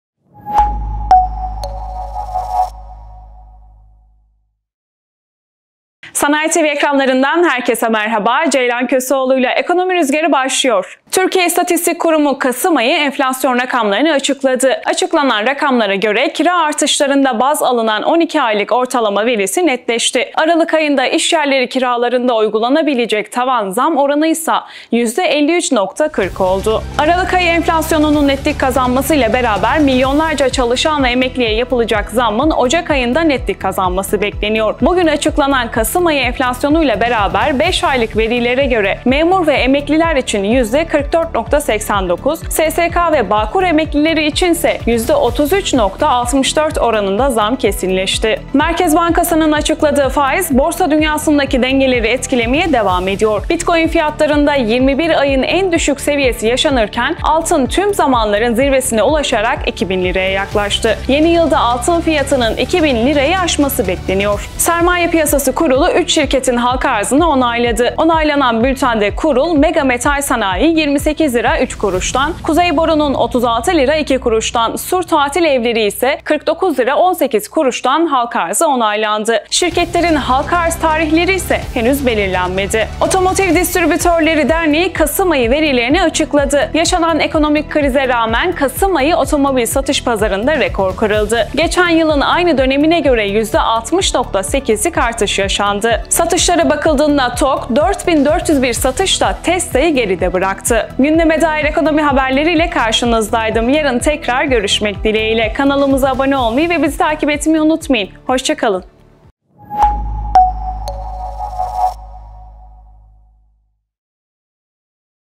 Emekli maaşlarında zam oranı netleşti!
Ekonomi Rüzgarı gündeme özel en sıcak haberler ile devam ediyor: Enflasyon Rakamları Açıklandı, Emekli Maaşı Yüzde 45 Artacak, Altın Rekora Kırdı, 3 Şirket Halka Arz oldu, TOGG TESLA’yı Süpürdü!.
Dünya ve Türkiye gündemine özel ekonomi haberlerini sizler için bir araya getirdim. Ben Ceylan Köseoğlu, Ekonomi Rüzgarı ile karşınızdayım. İyi Seyirler...